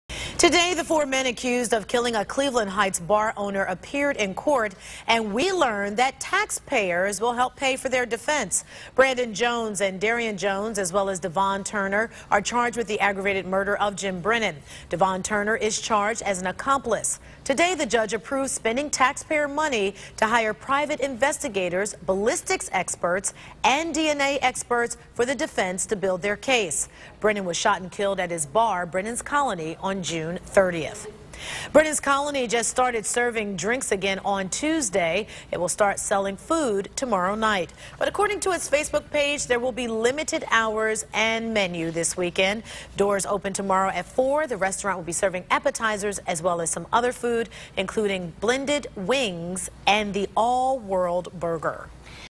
5PM: Suspects in Jim Brennan's shooting death appear in court
Four men accused of being involved in the shooting death of a prominent Cleveland Heights restaurant owner appeared in court Thursday.